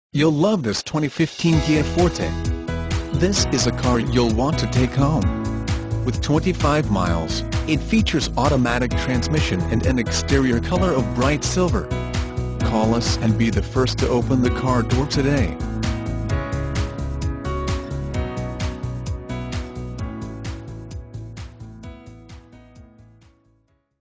2015 KIA FORTE Austin, TX 288104
2015 KIA FORTE Austin, TX
Stock# 288104

Capitol Kia
13573 North Hwy 183

You'll love this 2015 Kia Forte. This is a car you'll want to take home. With 25 miles, it features Automatic transmission and an exterior color of Bright Silver. Call us and be the first to open the car door today!